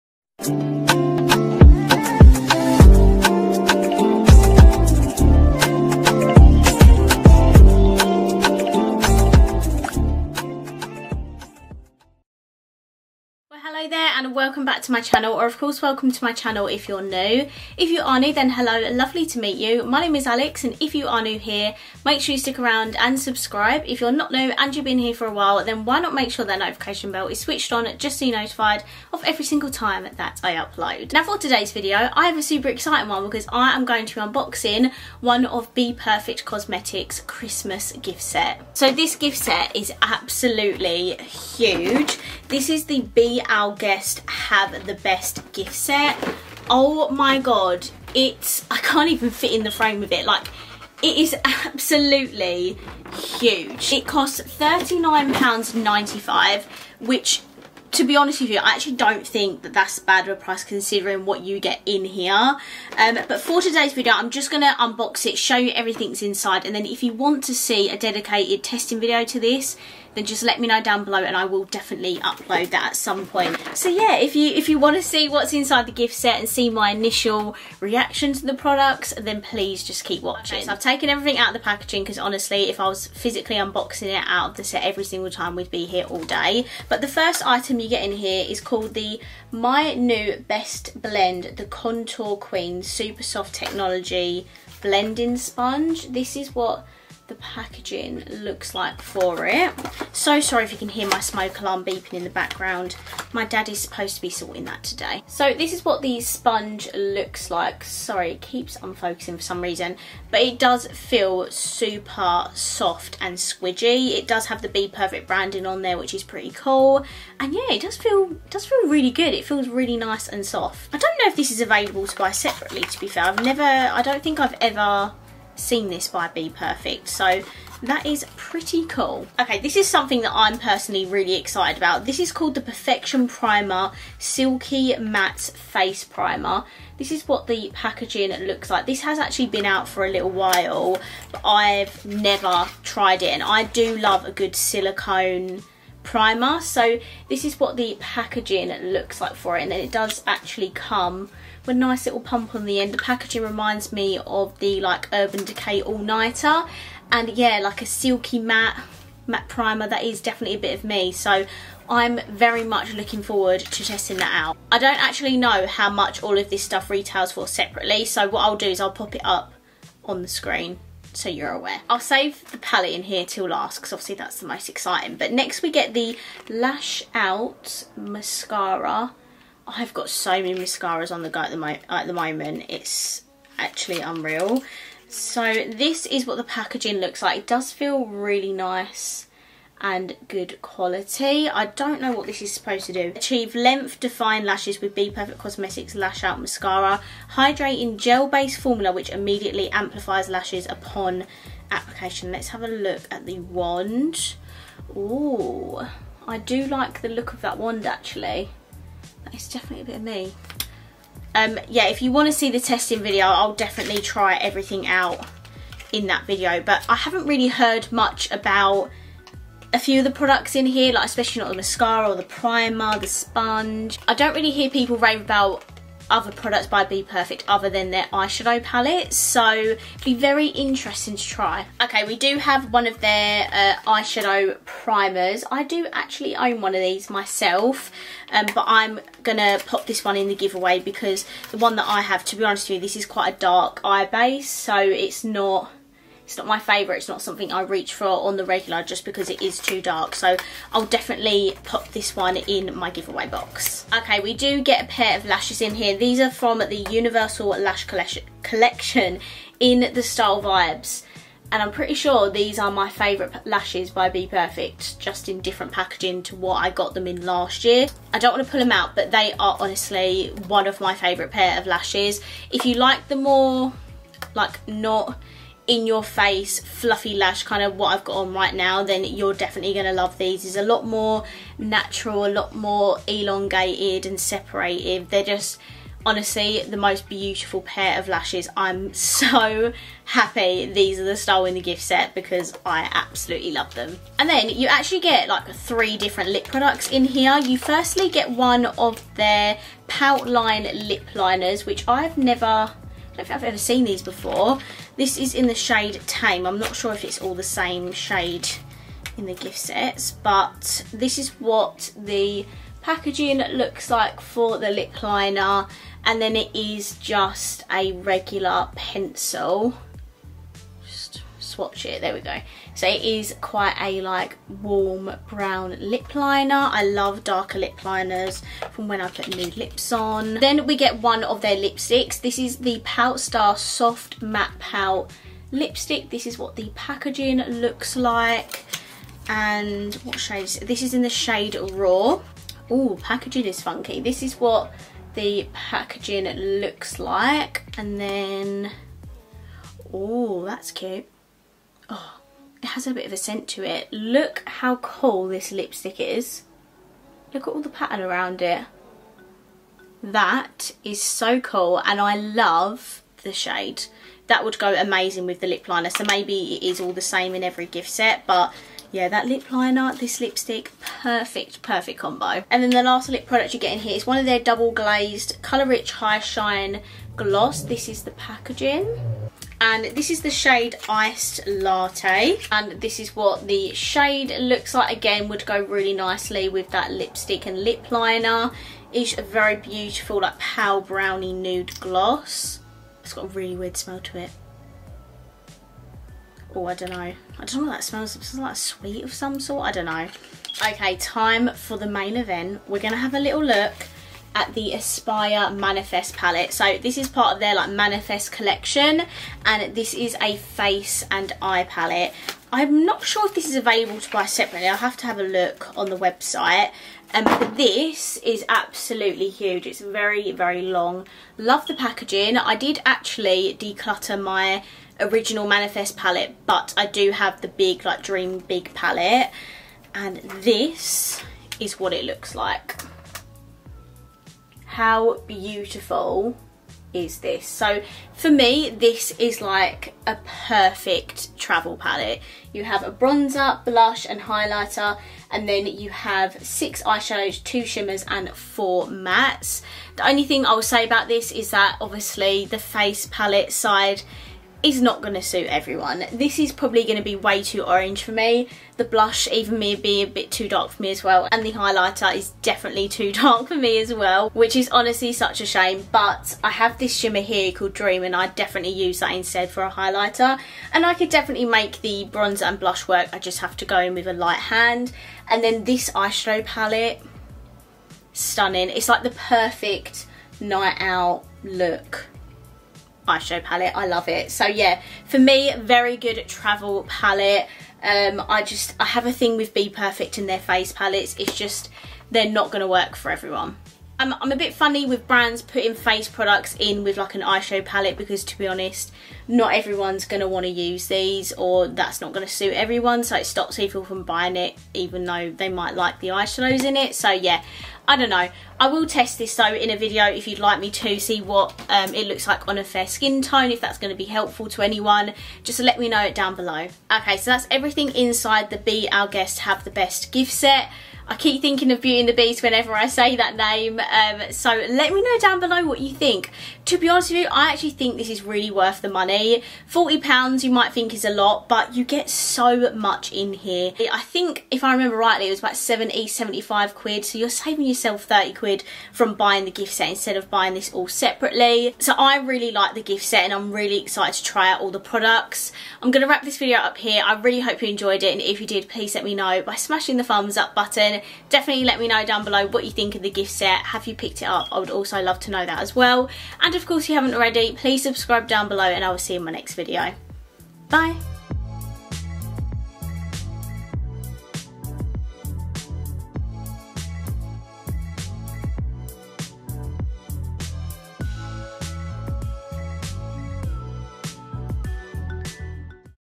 BPERFECT BE OUR GUEST HAVE THE BEST GIFT SET UNBOXING | makeupwithalixkate
Welcome to todays video where I am unboxing the Bperfect Be Our Guest, Have The Best gift set, I hope you enjoy!xo